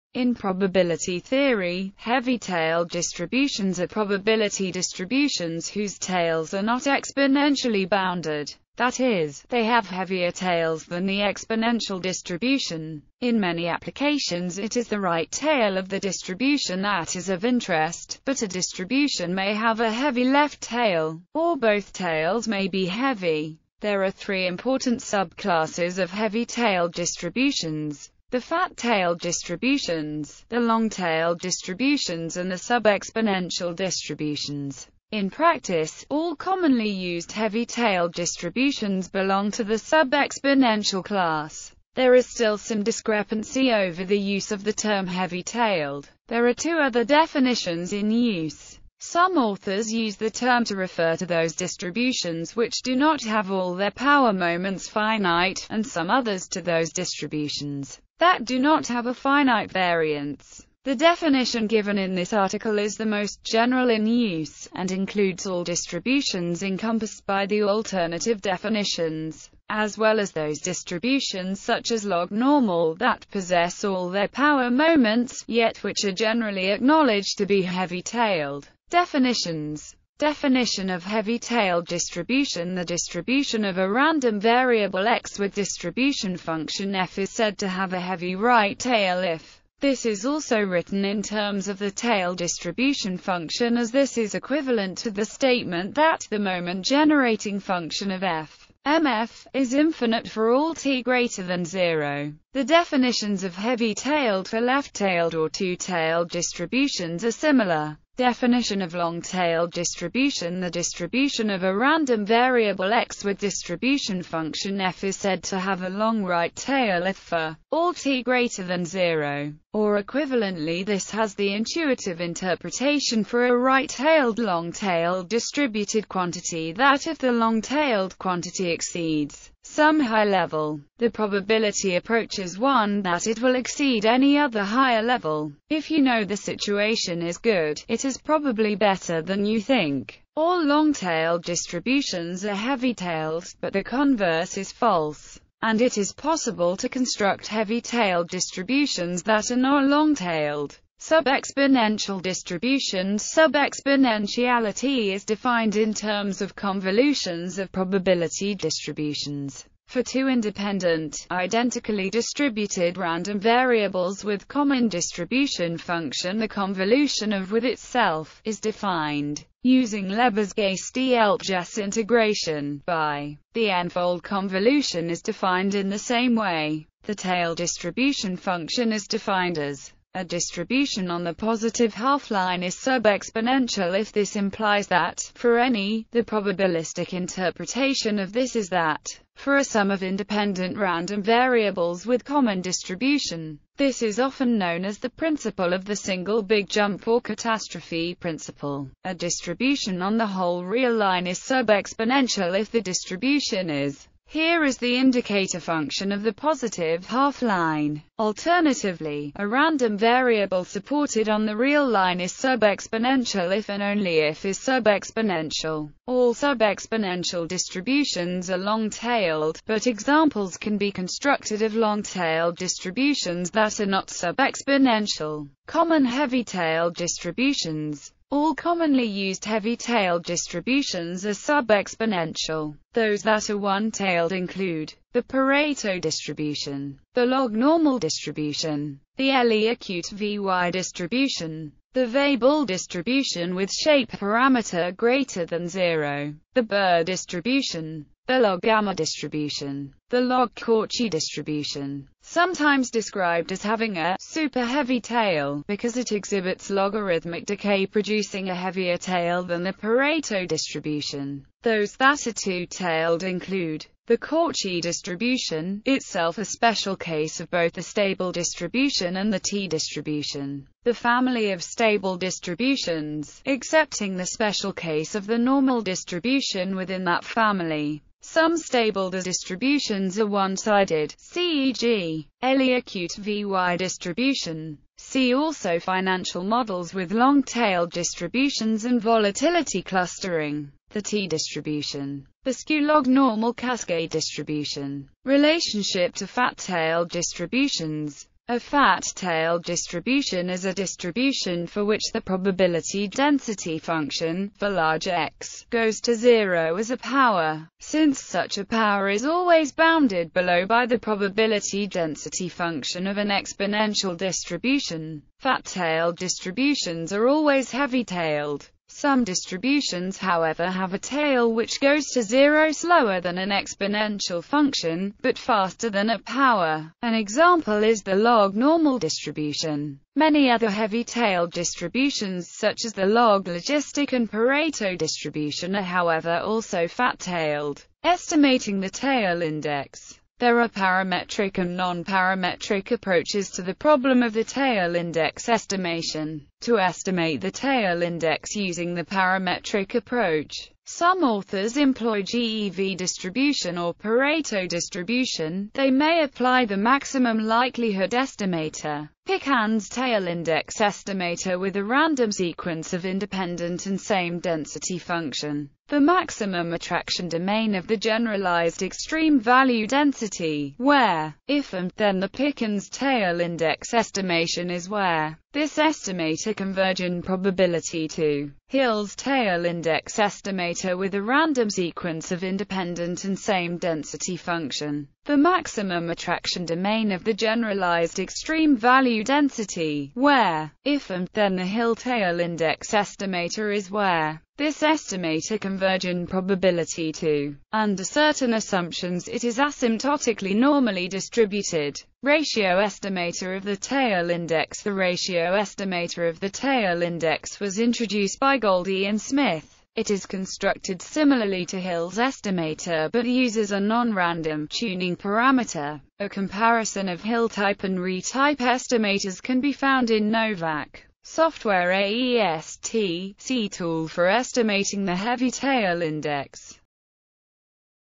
Heavy-tailed distribution
In probability theory, heavy-tailed distributions are probability distributions whose tails are not exponentially bounded: that is, they have heavier tails than the exponential distribution.In many applications it is the right tail of the distribution that is of interest, but a distribution may have a heavy left tail, or both tails may be heavy.